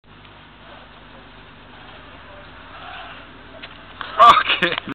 jordy trys the tranzfer
jordy trys to transfer and nearly wipes me out